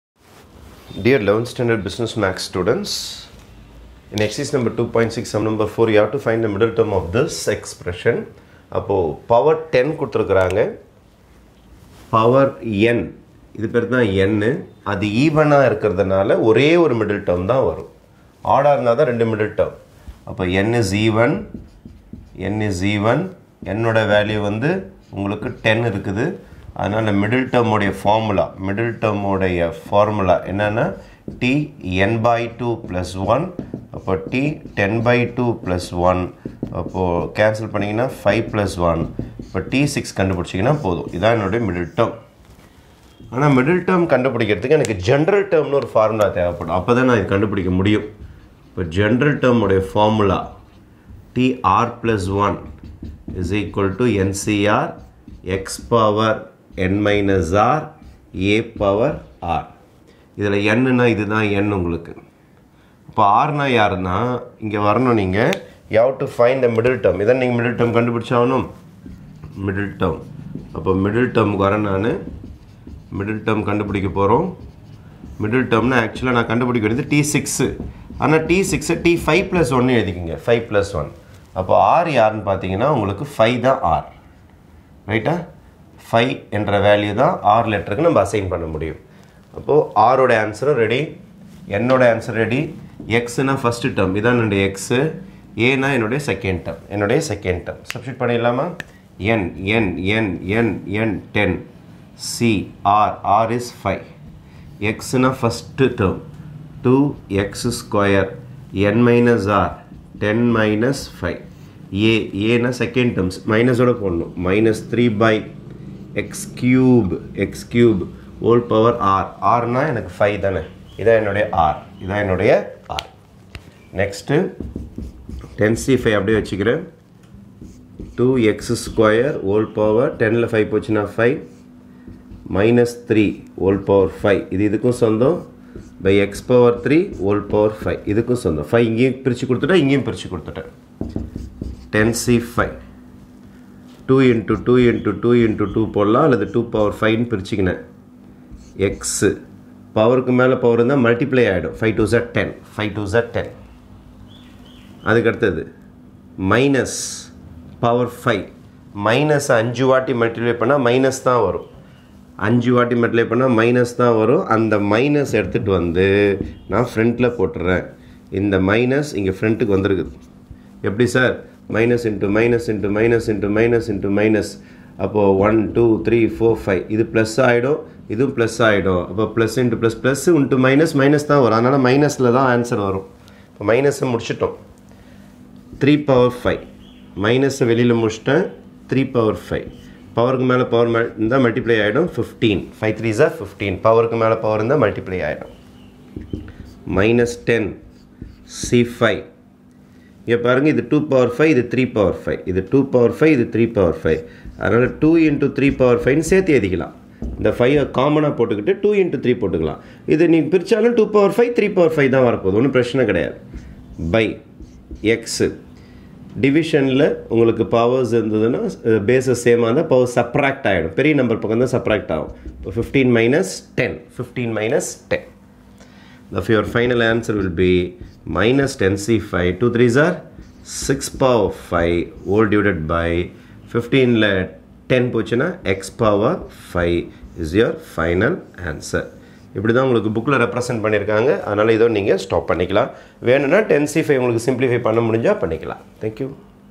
11th Std BM Ex.2.6(4)(iii) Find the middle terms in the expansion (2x^2-3/x^3)^10
11th STD BUSINESS MATHS

TAMILNADU SYLLABUS